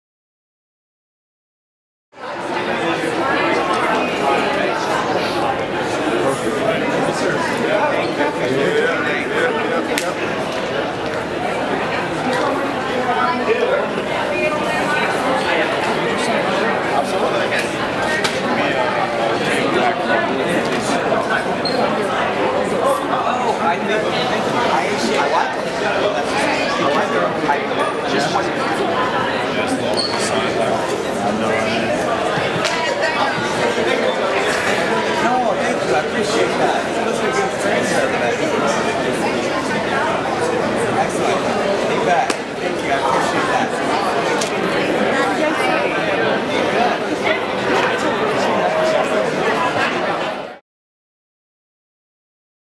Al Auguste - 2012 Meet the Olympians
Al Auguste at the 2012 Mr. Olympia Meet the Olympians in the Orleans Hotel in Las Vegas, Nevada.  For more bodybuilding news and information, check out: